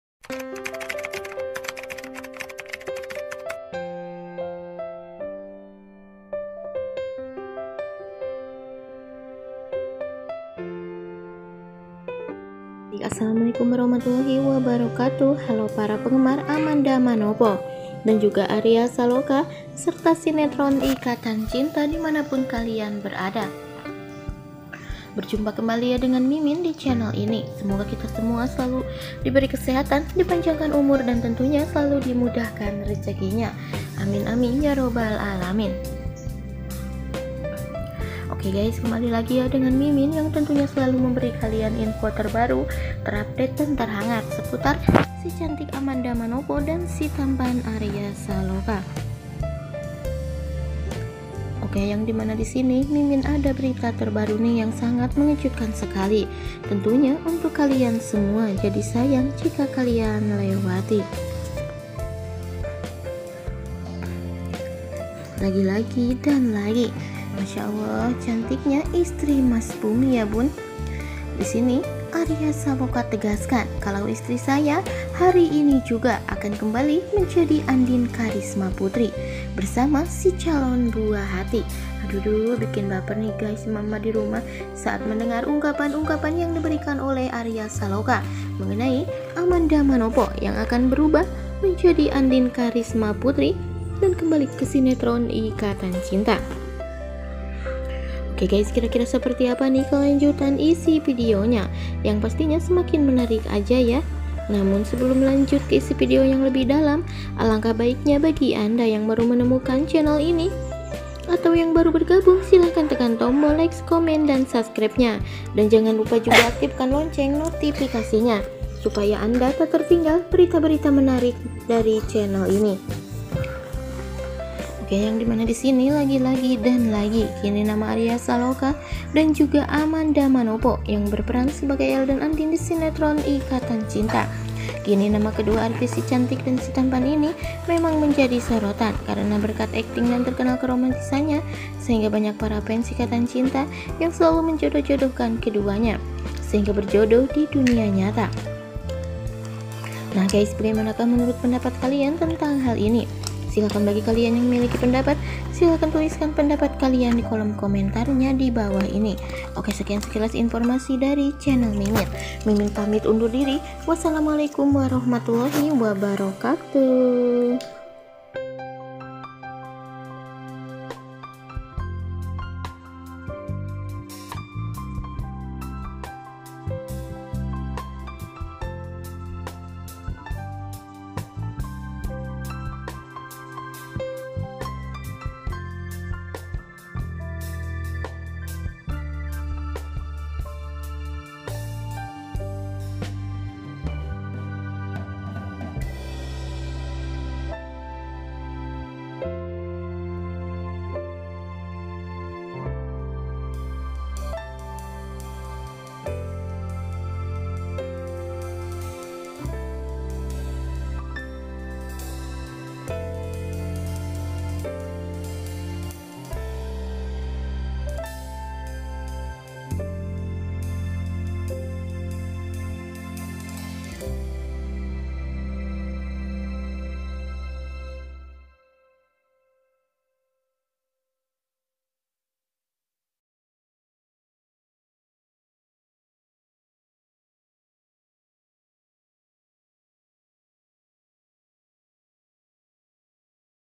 Heboh Arya saloka tegaskan kalau saat ini juga Andin karisma putri istri saya kembali ke IC...
__SEKIAN & TERIMAKASIH___